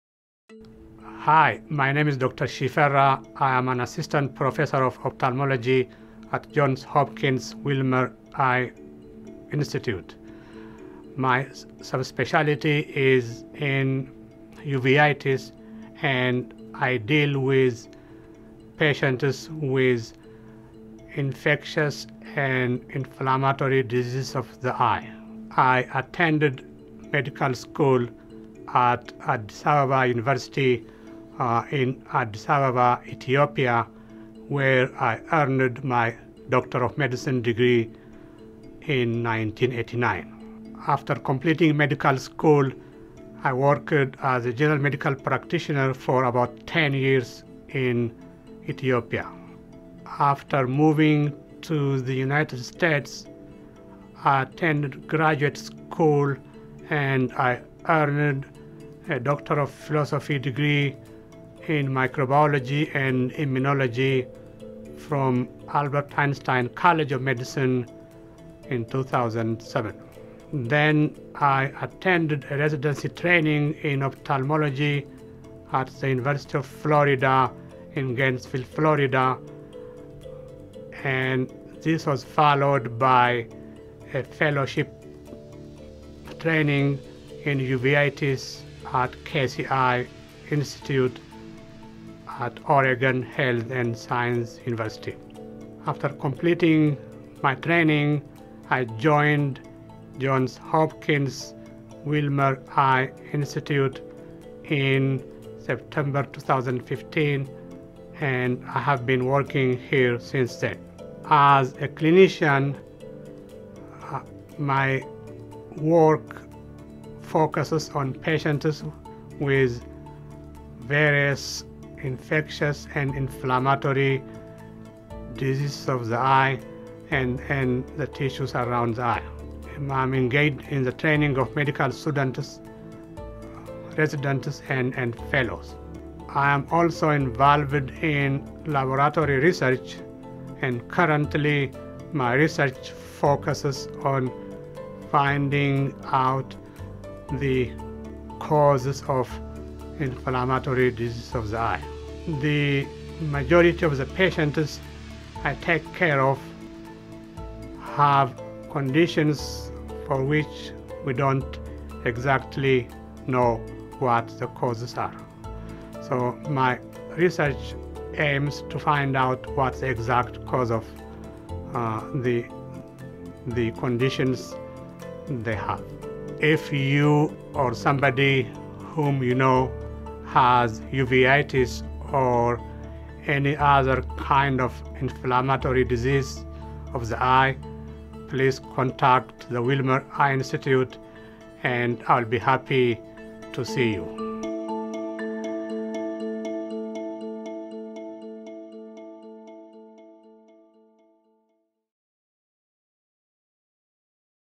Dr. Almde Selassie Shifera | Ocular Immunology
Amde Selassie Shifera, M.D., Ph.D., is an assistant professor of ophthalmology at the Wilmer Eye Institute. In addition to comprehensive ophthalmology, he specializes in ocular immunology, including uveitis, panuveitis, scleritis and ocular inflammatory and infectious diseases. His research focuses on investigating the causes of uveitis and in developing new and better means of treatment for uveitis.

Dr. Shifera received his M.D. from Addis Ababa University in Ethiopia. After ten years of clinical practice in Ethiopia as a general practitioner, he moved to the United States and completed a master’s degree in clinical chemistry at West Chester University and then a Ph.D in microbiology and immunology at the Albert Einstein College of Medicine. Following postdoctoral research fellowships at both Albert Einstein and the University of California, San Francisco, he completed an internship in surgery at the University of Colorado and then his residency in ophthalmology at the University of Florida. Prior to joining the Wilmer faculty in 2015, Dr. Shifera completed a clinical fellowship in uveitis at the Casey Eye Institute at Oregon Health & Science University.